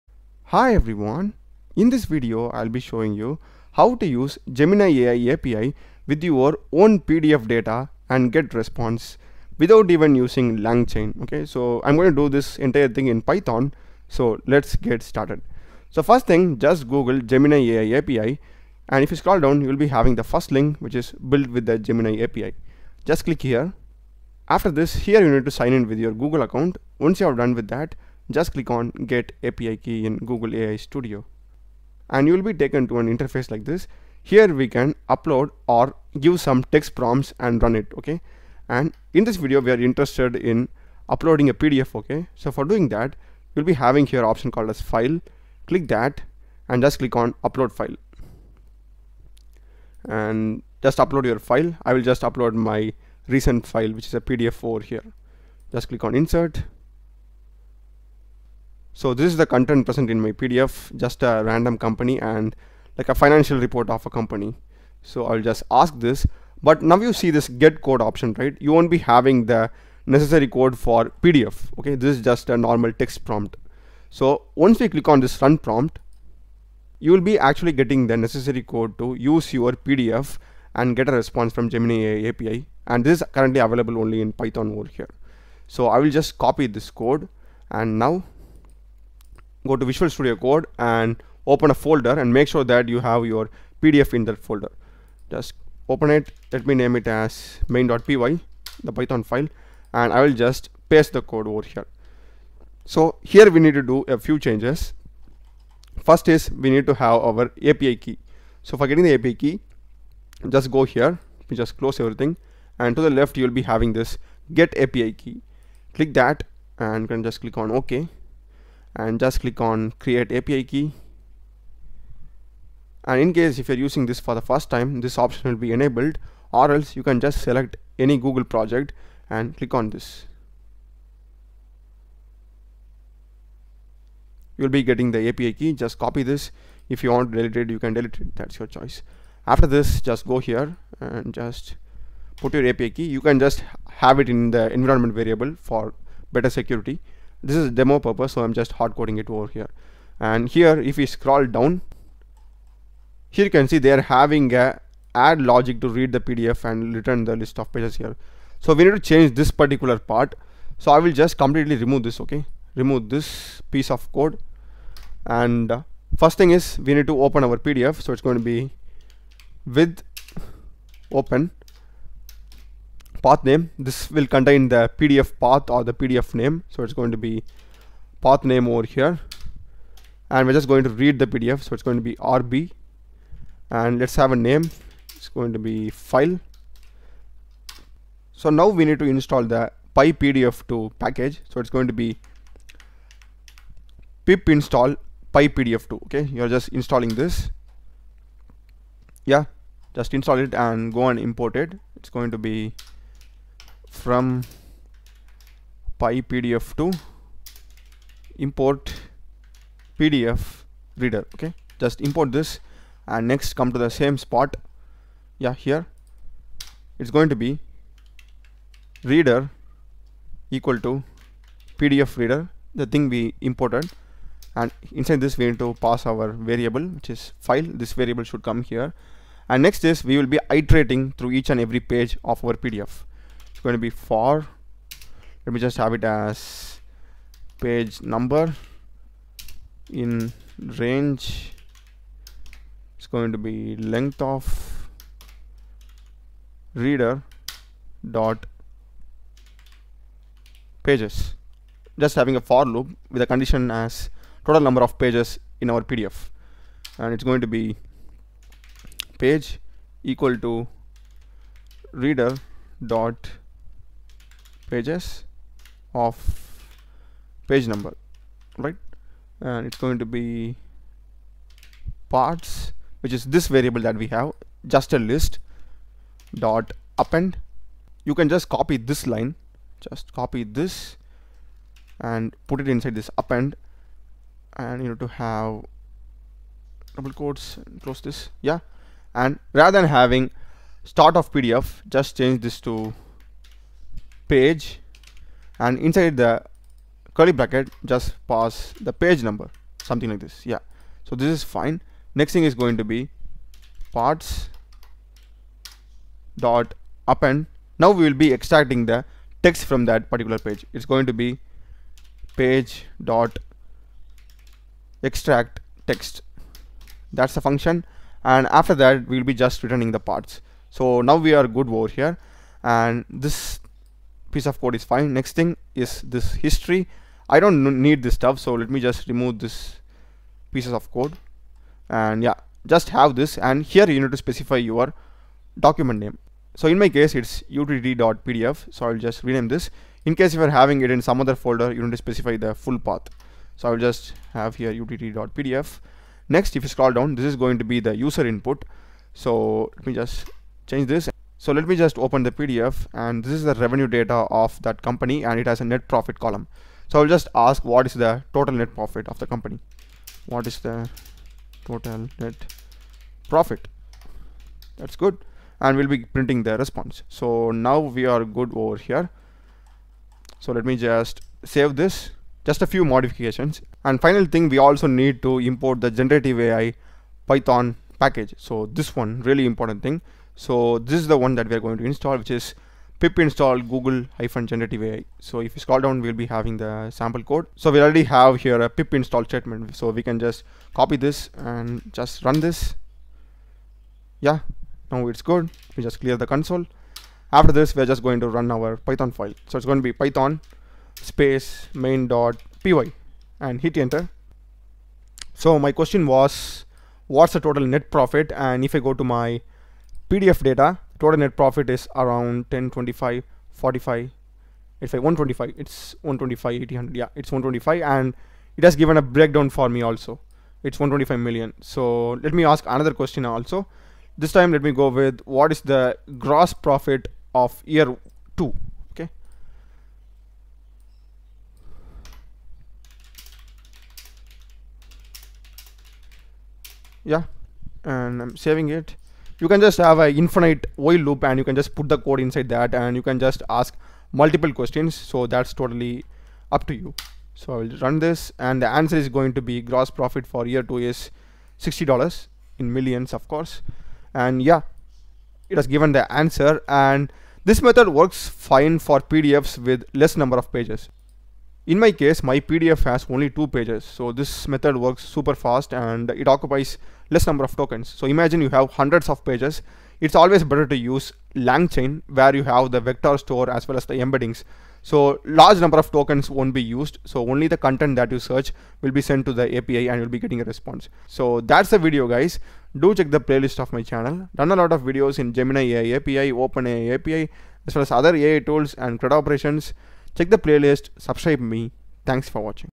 Chat with your PDF using Gemini AI API in Python without Langchain
In this tutorial we will see how to use gemini ai api with your own custom pdf document data and chat by asking questions to it without using langchain in python